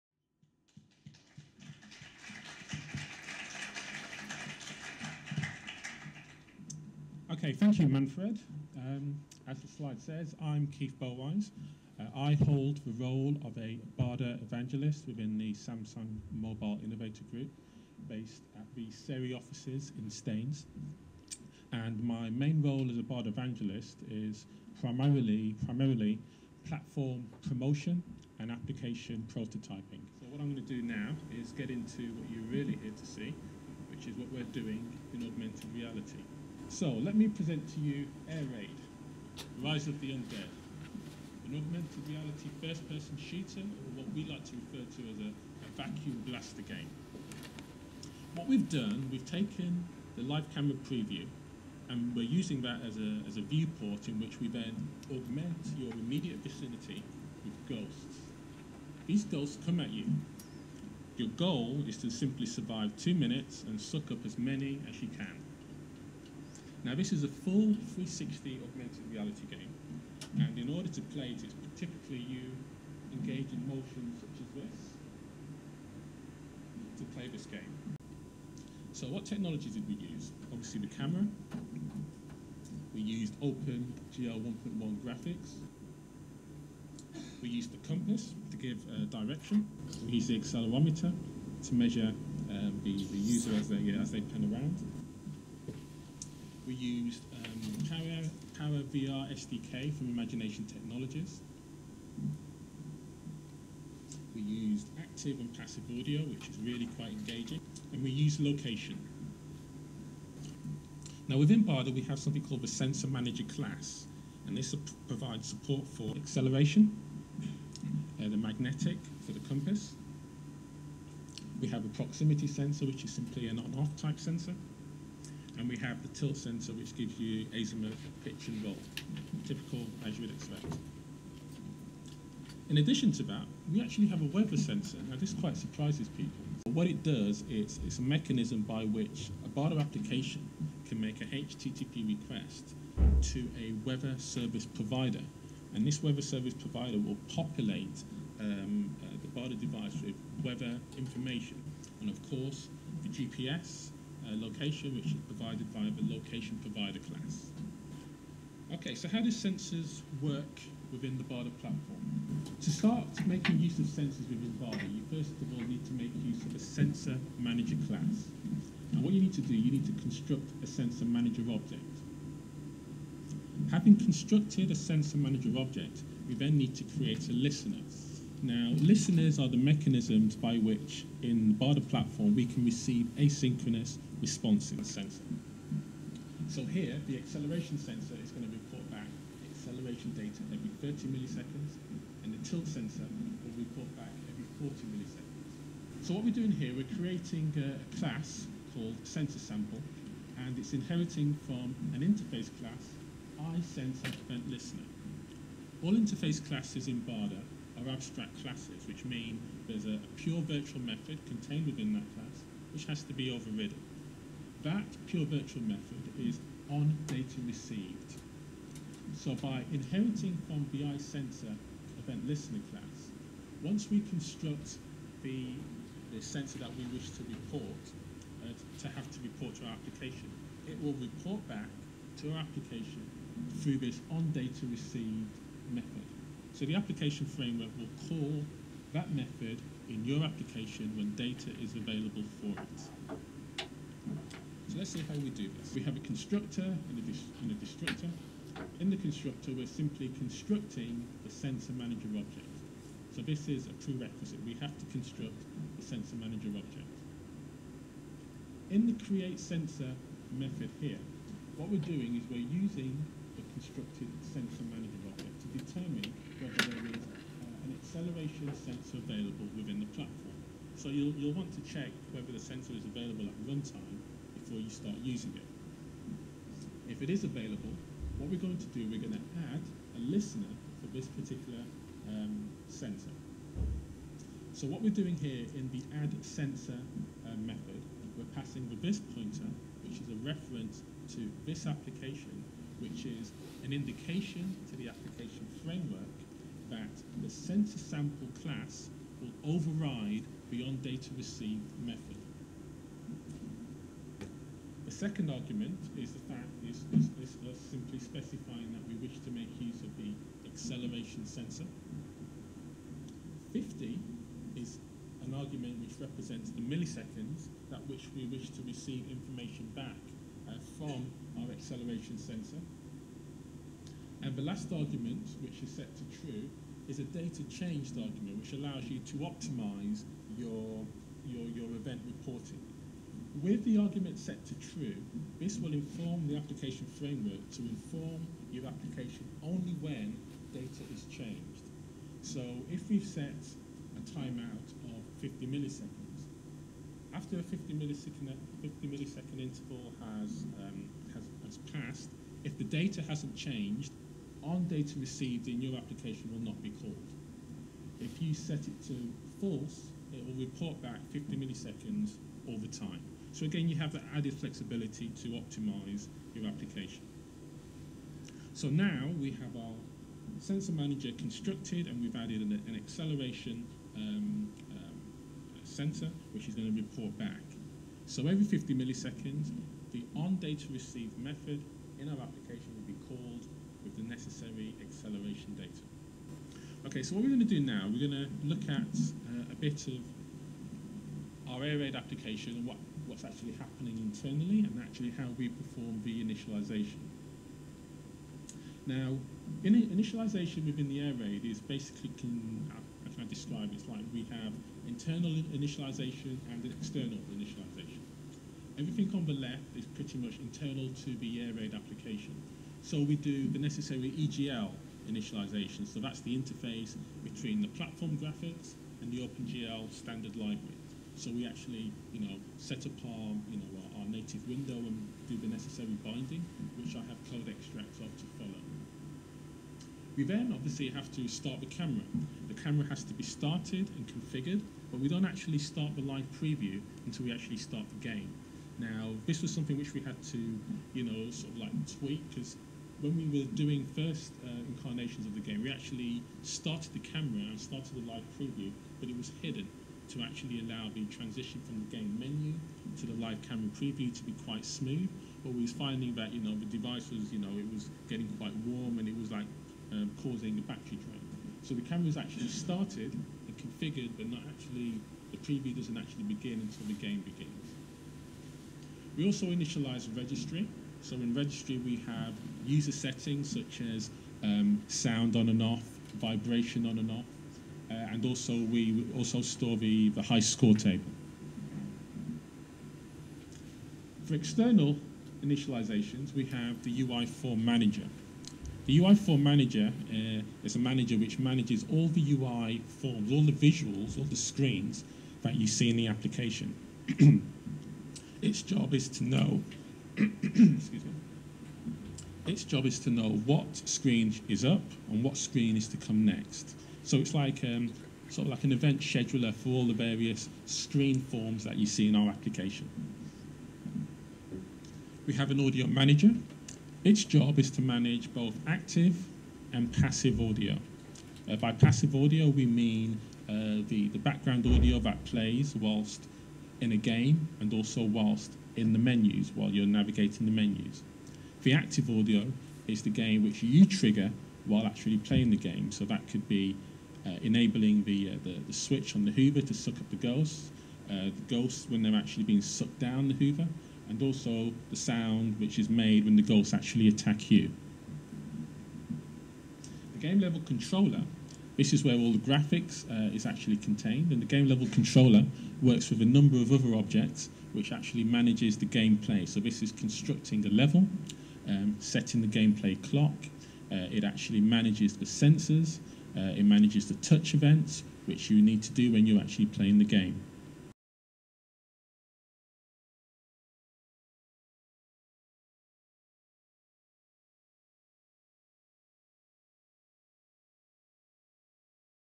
Augmented Reality on bada [pt1] @ AP2011
This video showcases Keith Bauwise, bada evangelist, who was honored to speak at the annual Augmented Planet (AP2011) event on June 1st held at Kensington Town Hall, London. Samsung being Gold sponsors of the event, took the opportunity to share their work in the area of augmented reality (AR) by providing insights into their very popular bada AR game that is AiRaid -- Rise of the Undead.

The presentation has been split into three videos,  showing what mobile technologies were utilized to create the game.
A discussion on what sensors are present within the platform and how they can be used is provided along with code examples. In addition to this, explanations are given regarding how the graphics platform is initialized, how the layering is achieved to produce OpenGL graphics, over a live camera preview and also the internal working of the game play graphical engine.

The presentation ends with Keith proposing the new developments for the AiRaid game and also exciting extensions to the bada SDK (AiRaid library) that will allow other bada developers to quickly realize their AR applications based on the work already performed in AiRaid.